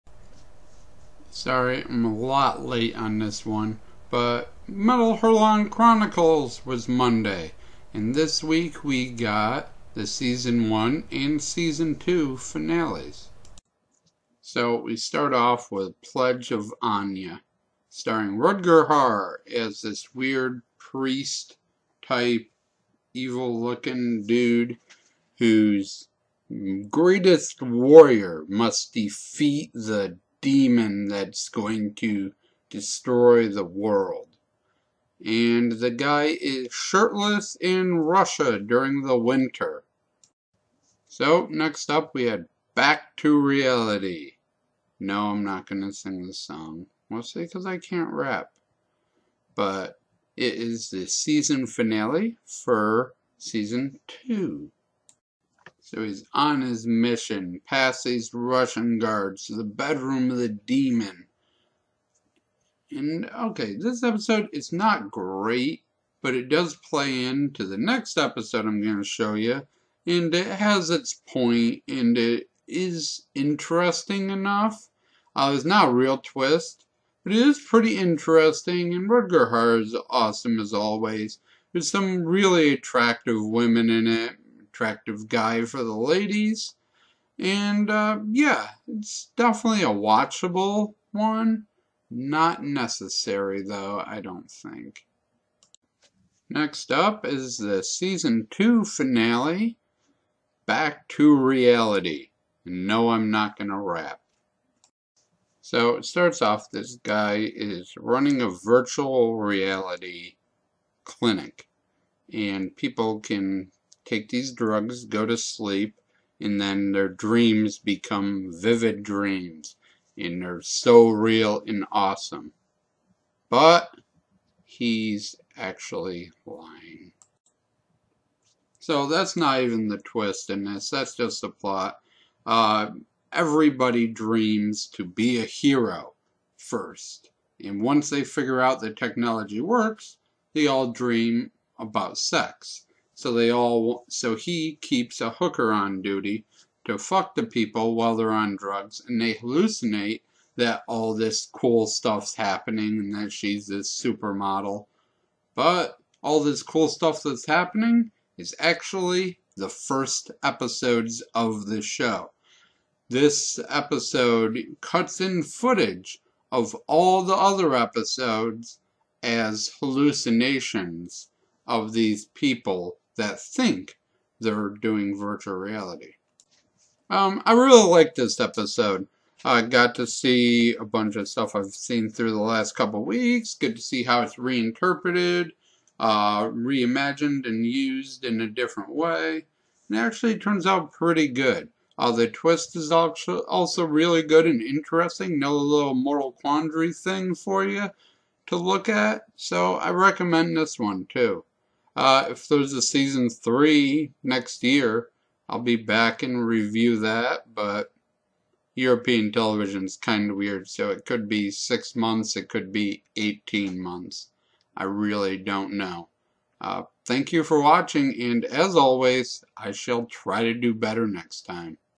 Review of Métal Hurlant Chronicles season finales by Cornerstore
Review Métal Hurlant Chronicles episodes 1.6 and 2.6